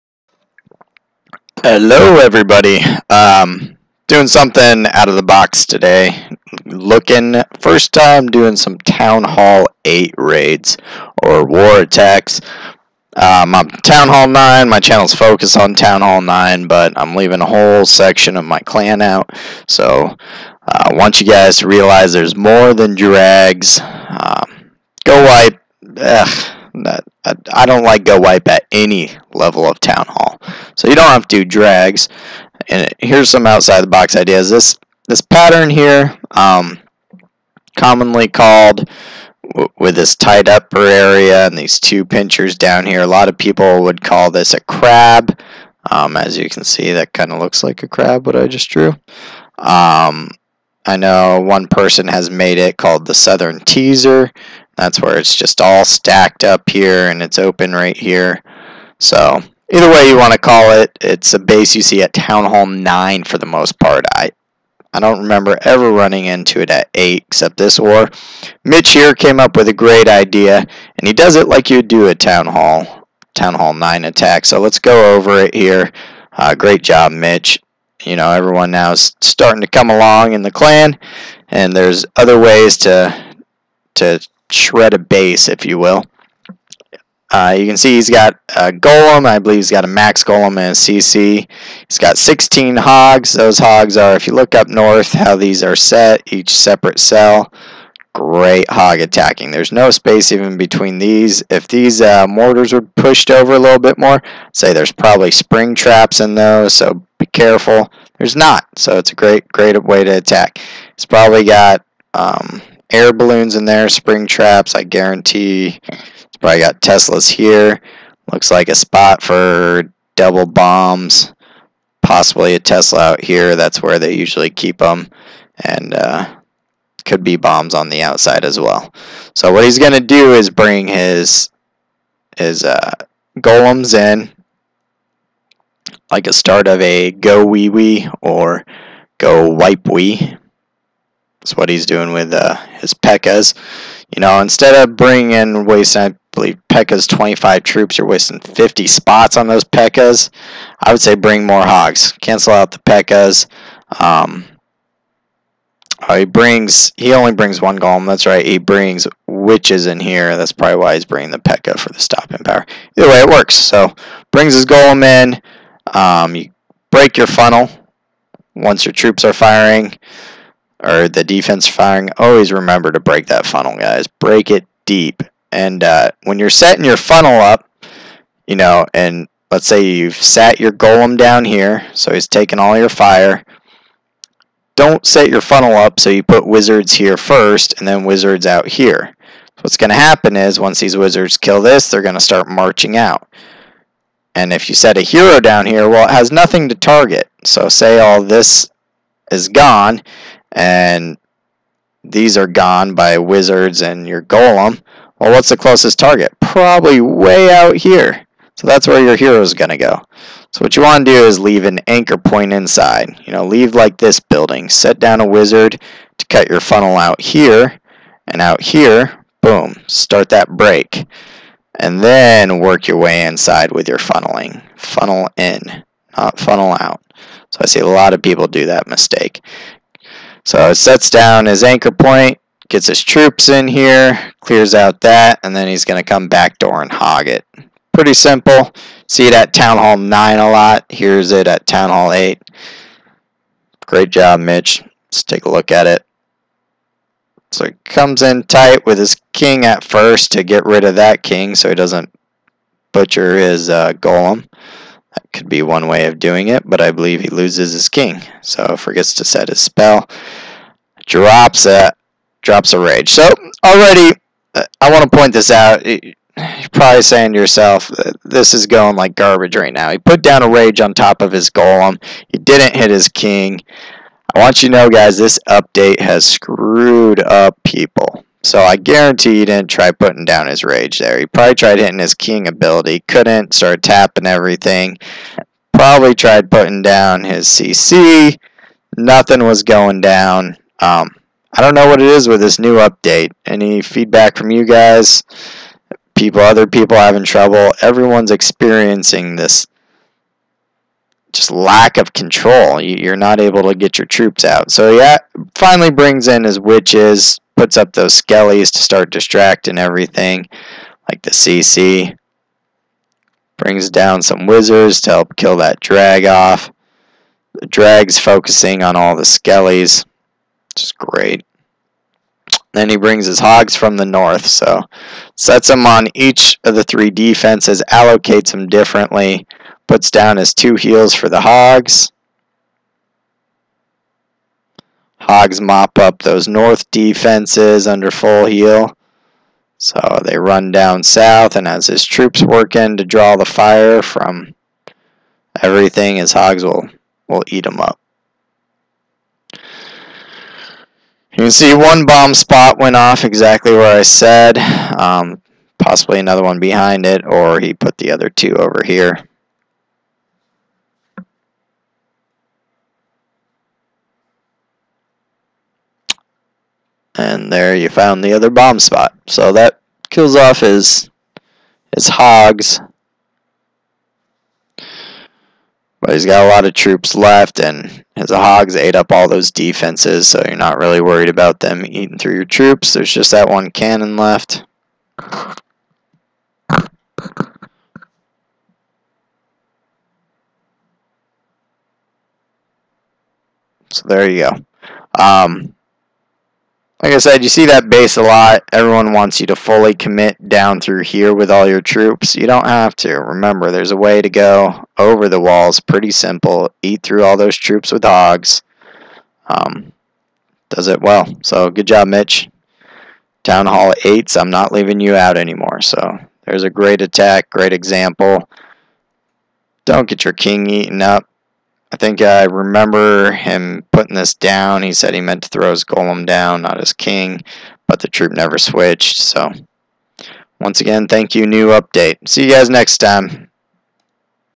GoHoWiWiPe by Mitch. TH8 "crab/southern teaser" 3 STAR
here is a base usually seen built at TH9. Mitch does a hybrid attack for a 3 star.